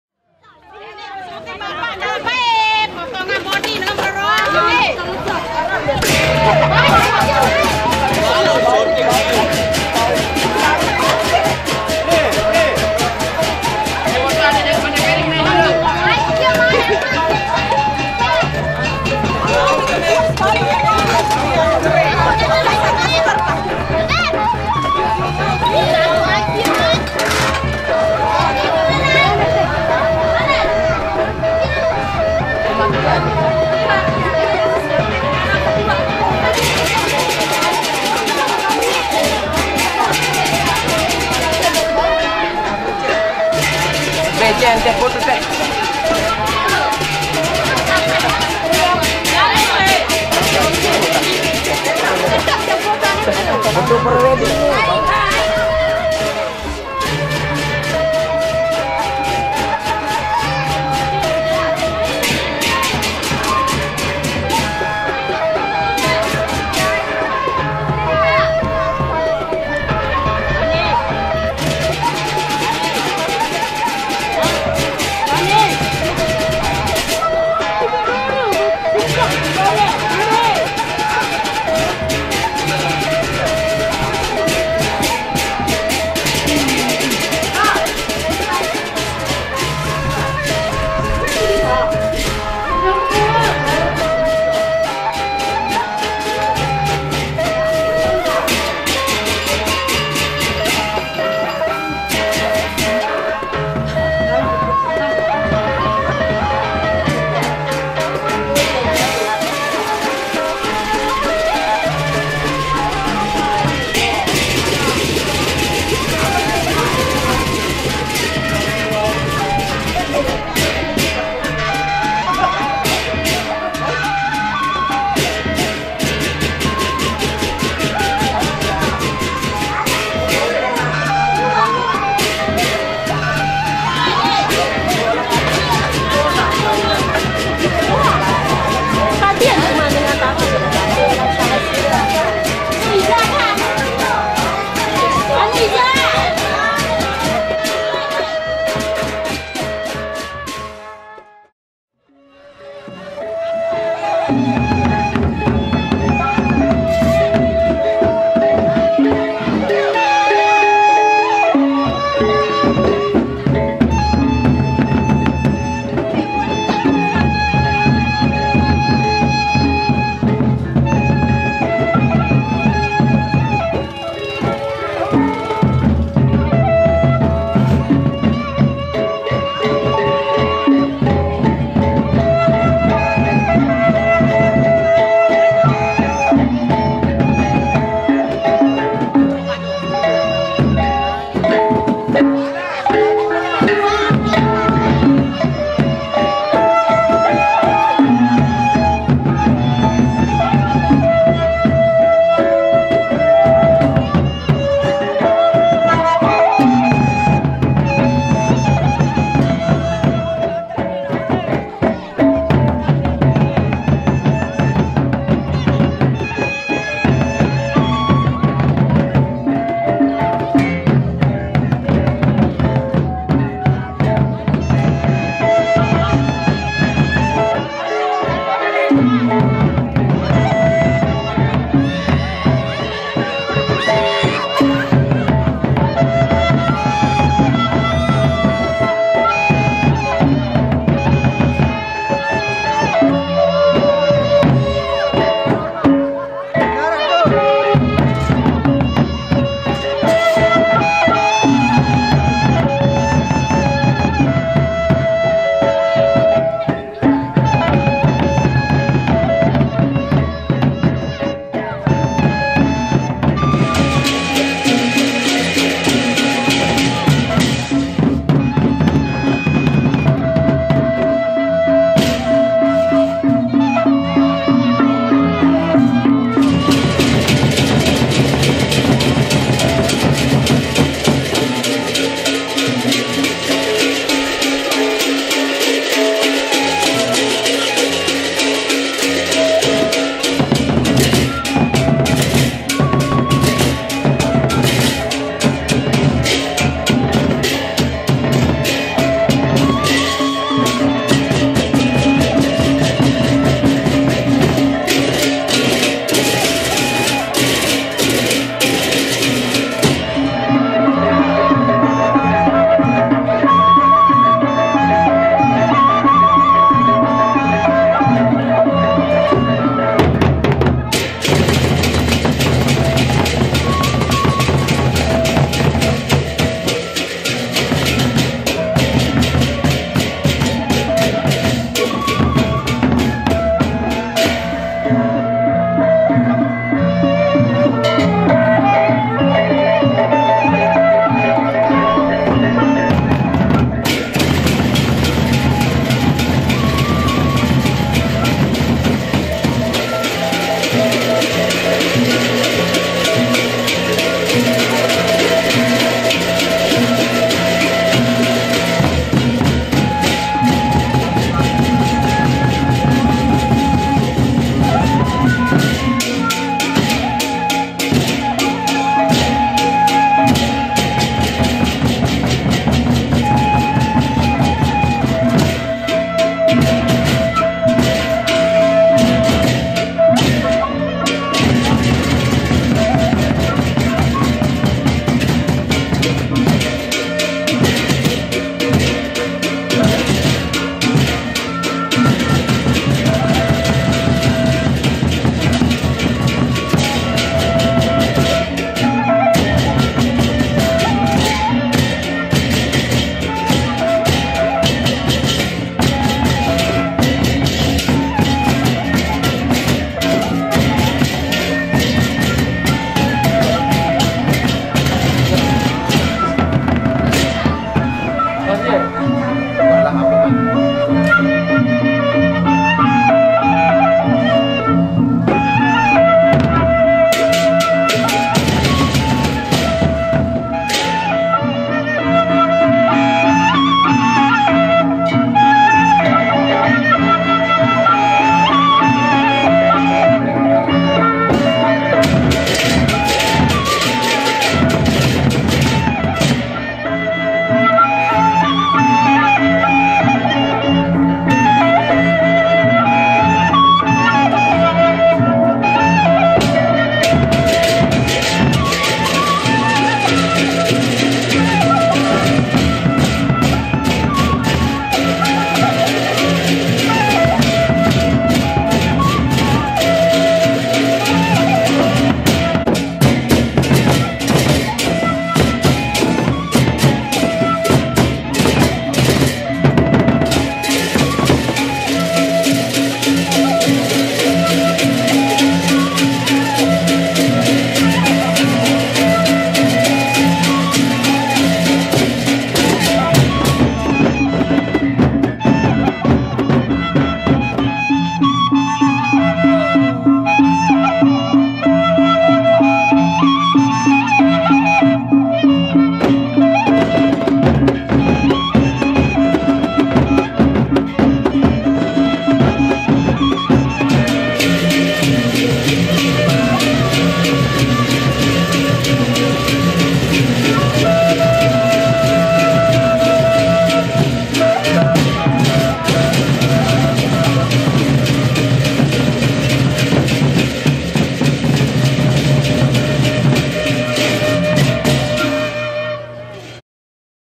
TABUH GEMURUH GENDANG BELEK SAKRA Sampai Kakek2 Ikut Joget terpancing oleh cantiknya sang PENARI
KHASIMA